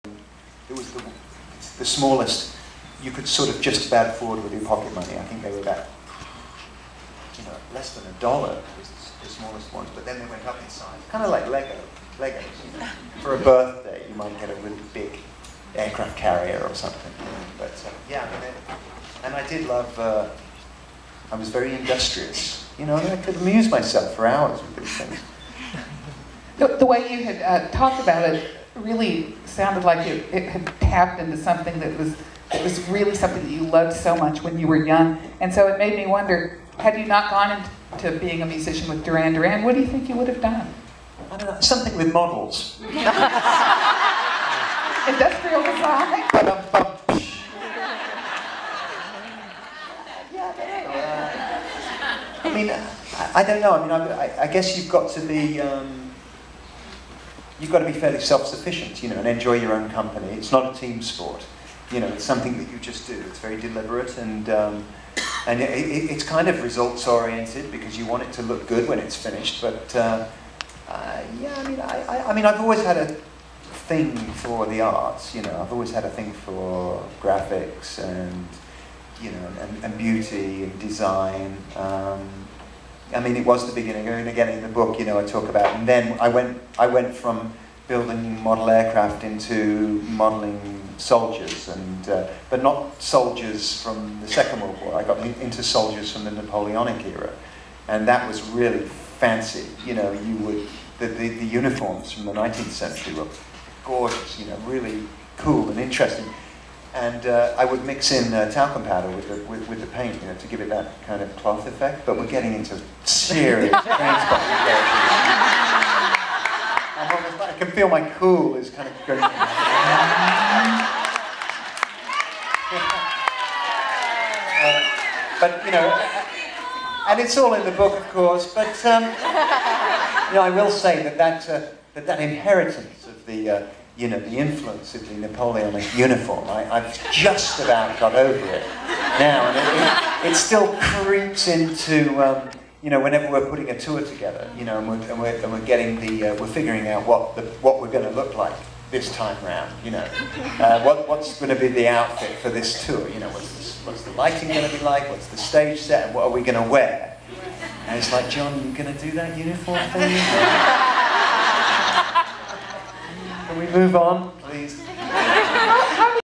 Two minutes with John Taylor
A snippet of John Taylor talking about childhood memories of building models as a child in England and how it influenced him later in life.  Taken at the Scottish Rite Theater, Austin TX.